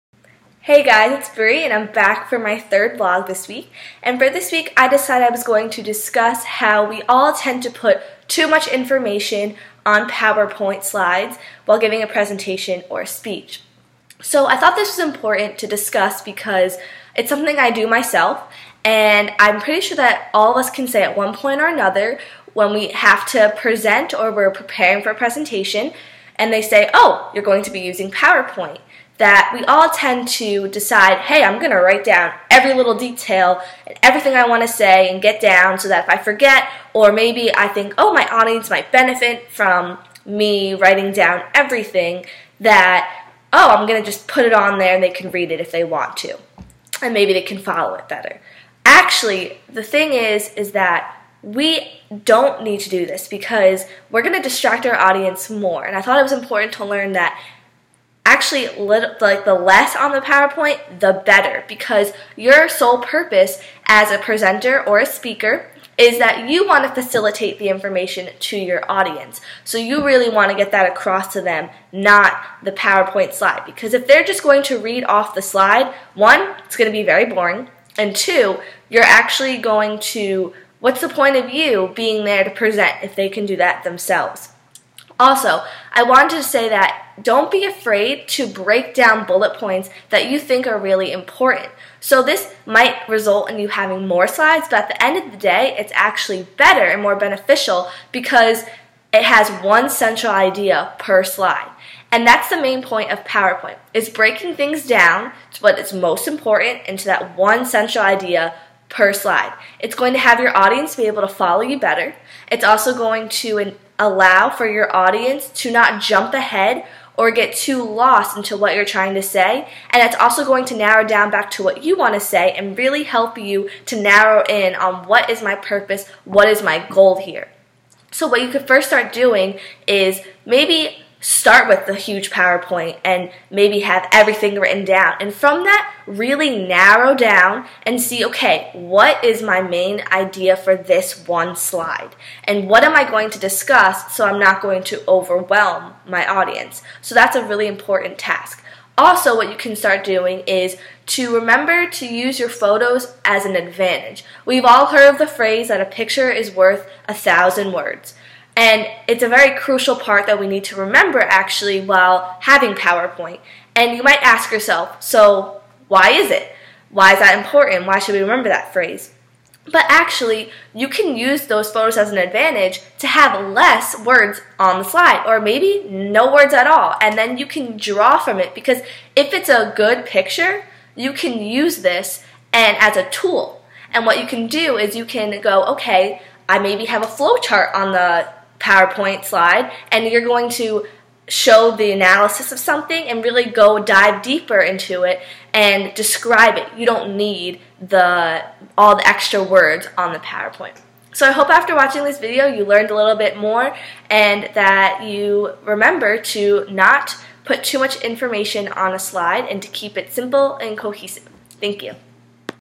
Vlog 3
This video is about how we all tend to add too much information on slides while using PowerPoint. I got my information from SlideGenius and this is also the link to the article. Enjoy!